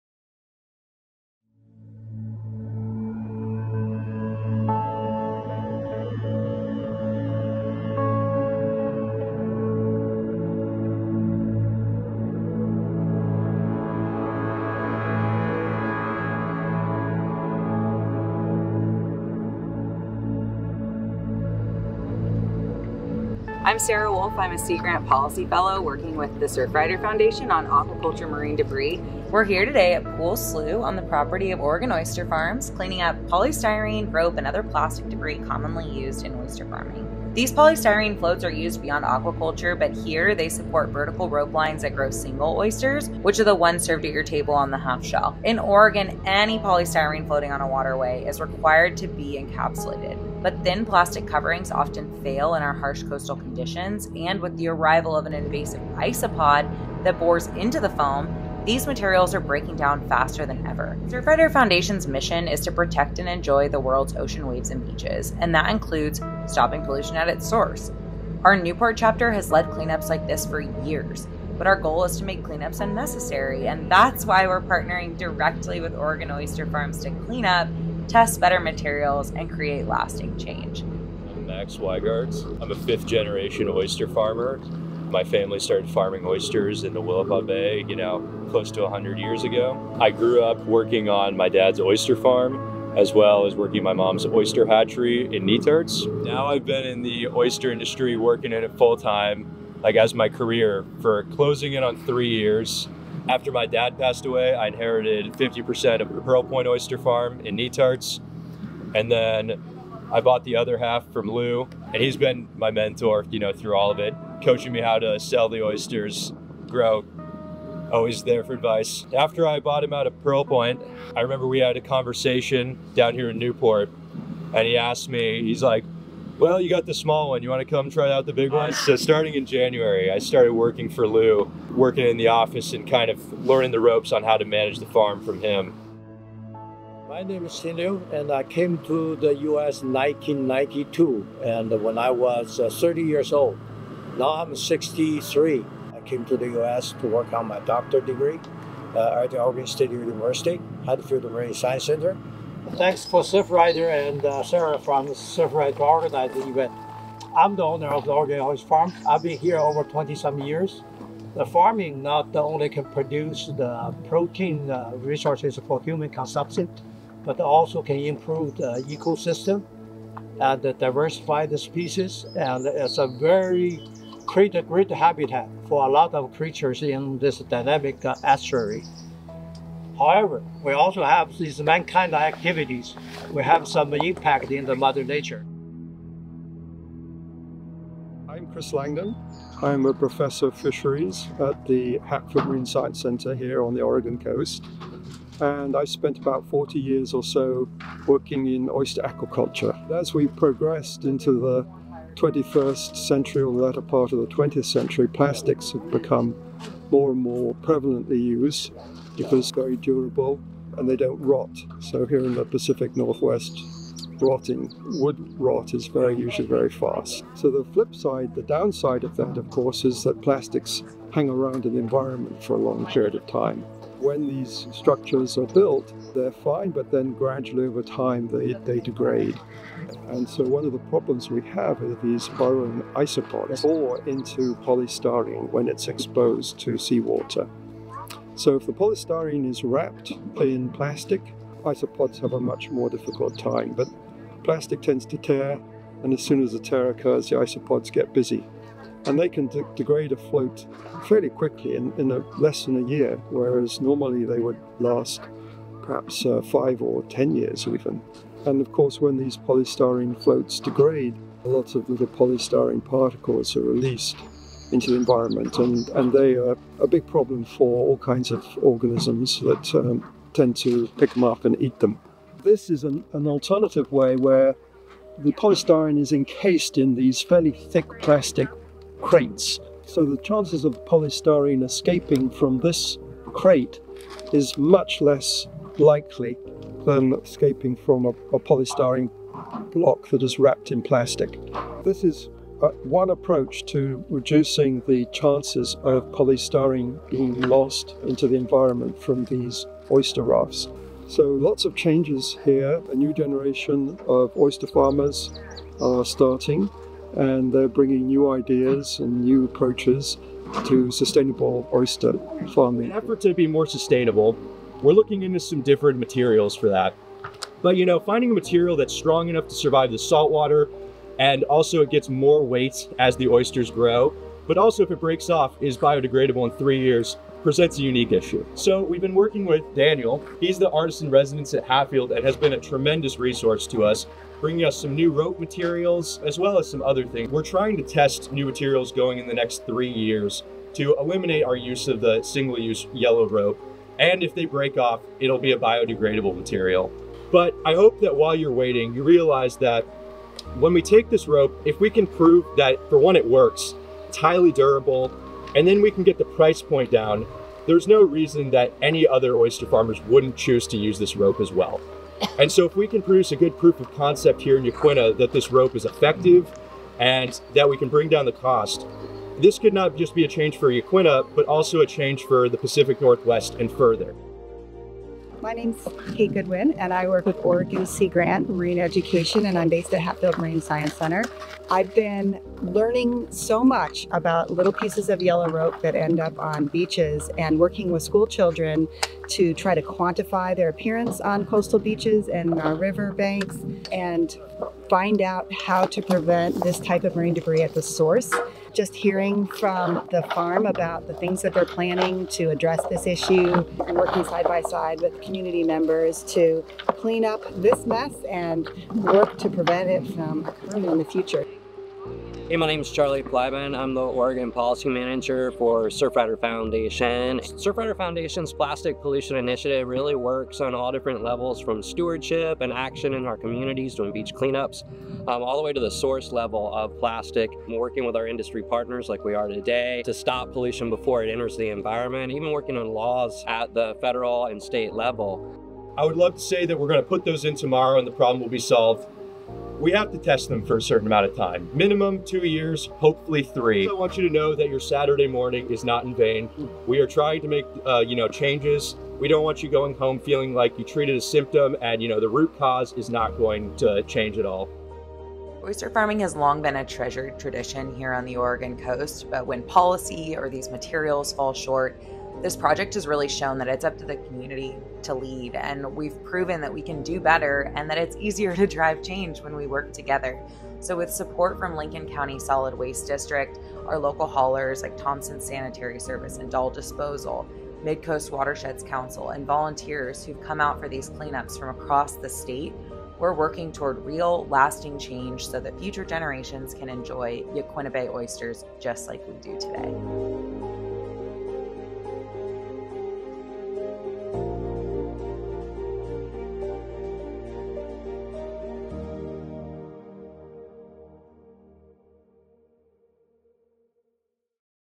Community Solutions for Aquaculture Marine Debris | Surfrider Foundation x Oregon Sea Grant
What happens when outdated aquaculture gear threatens the health of our coastal ecosystems?

This short documentary spotlights a community-driven response in Yaquina Bay, Oregon, where oyster farmers, scientists, nonprofits, and local residents are coming together to tackle marine debris caused by aging aquaculture infrastructure—like polystyrene floats and yellow rope.

📍 Filmed in Yaquina Bay, Oregon by CKC Media

Featuring:
➤ Dr. Sarah Wolf – Surfrider Foundation, Oregon Sea Grant
➤ Max Wiegardt – Owner of Pearl Point Oyster Co. & Manager at Oregon Oyster Farms
➤ Xin Liu – Owner, Oregon Oyster Farms
➤ Dr. Christopher Langdon – Oregon State University
➤ Cait Goodwin – Oregon Sea Grant
➤ Charlie Plybon – Surfrider Foundation
➤ Daniel Morgan – Skipjack Labs

This project was made possible by support from:
Builder’s Initiative, Surfrider Foundation, Oregon Sea Grant, Lincoln County Solid Waste District, Thompson’s Sanitary Service, Dahl Disposal, and the MidCoast Watersheds Council.